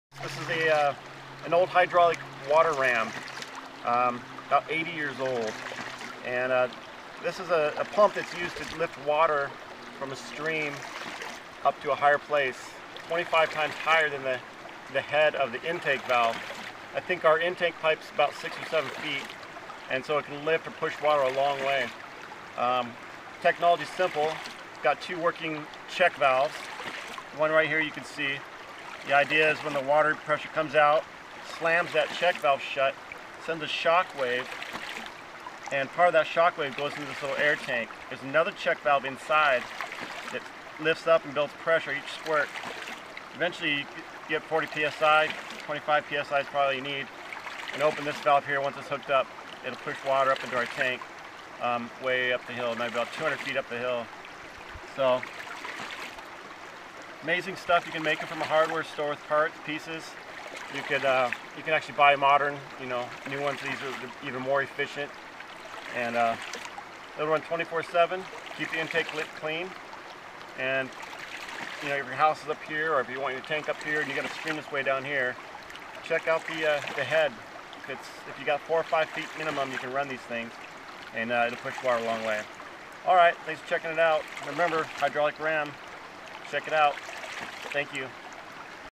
Rife Ram Pump
Water ram pump made by Rife. Used to pump water to a higher place. Uses no electricity, just two check valves and the falling water thru the drive pipe. Can push water 15-30 times higher than the head of the intake.